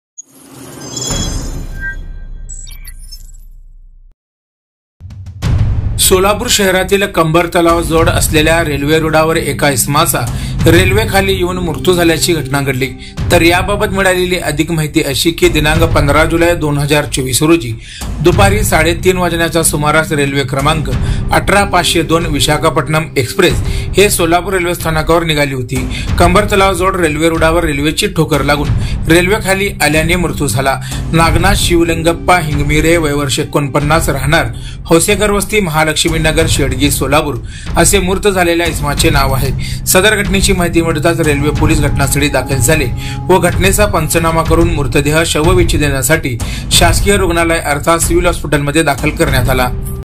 सोलापूर : कंबर तलाव जवळील रेल्वे रुळावर एका इसमाचा मृ*त्यू...
Lokshakti News